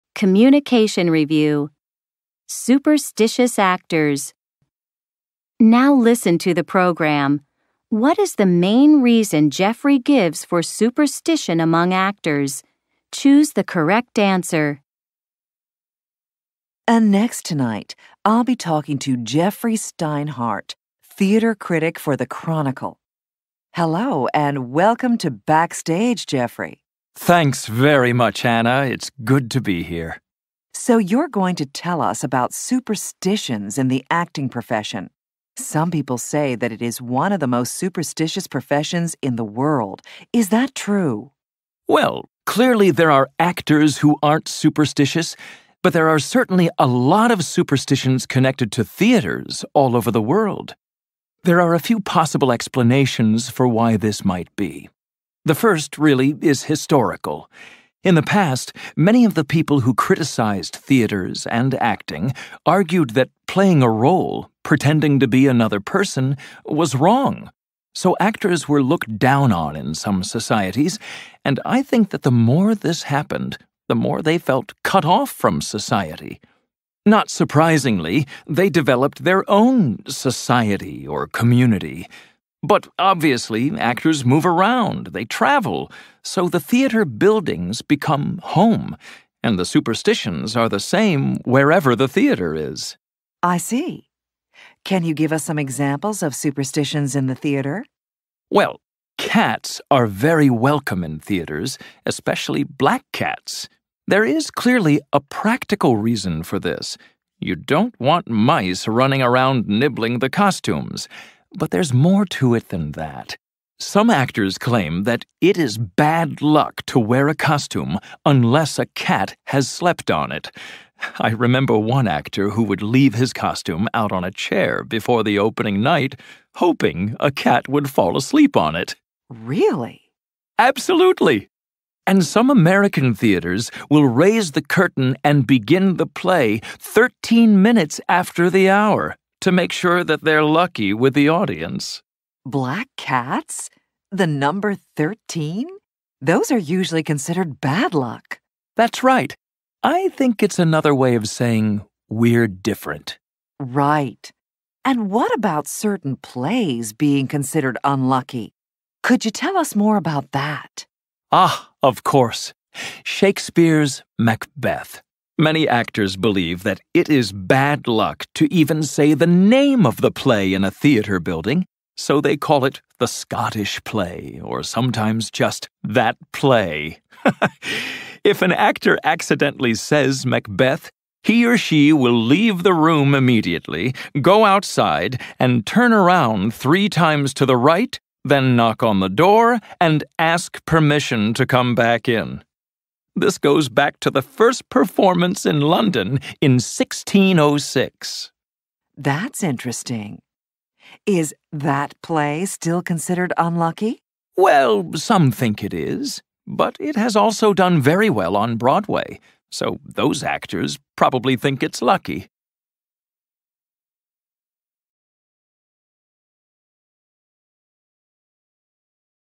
LISTENING Superstitious actors
🔘RuiEnglish Lec: Movies and television
L5U5PC3B Now listen to the program. What is the main reason Jeffrey gives for superstition among actors? Choose the correct answer.

⬜a. They used to feel isolated from the rest of society.
⬜b. They are insecure.
⬜c. They travel a lot